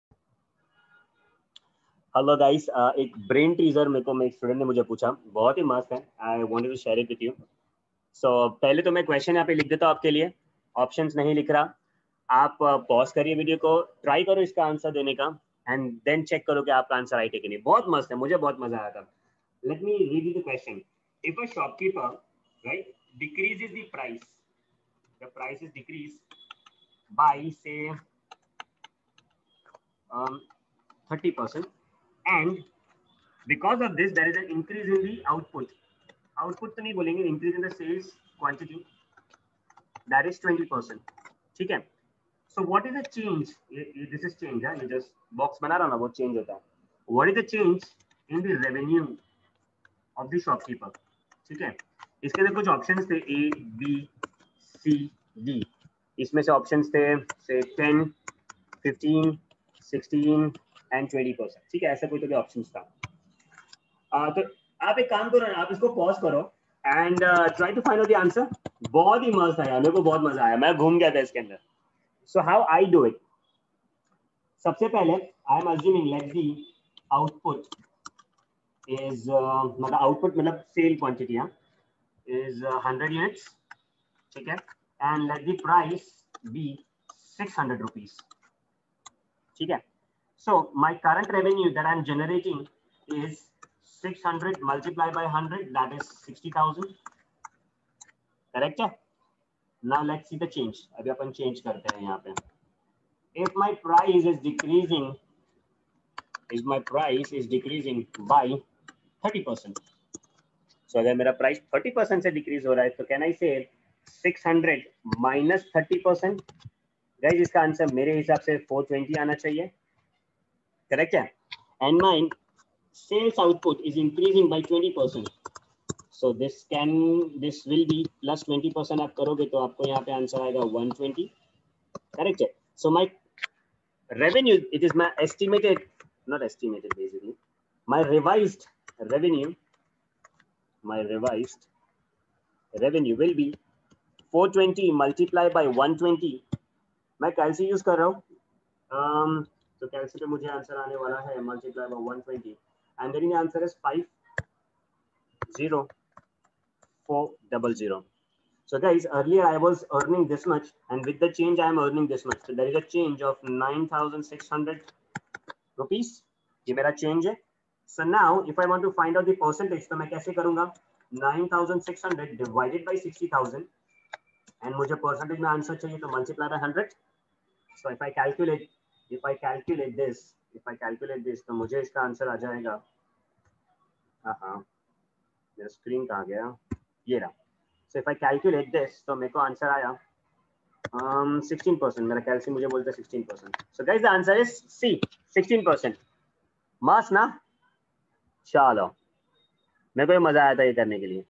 Sneaky math problem
This is the type of questions which proves how important it is to actually solve math problems rather than trying them mentally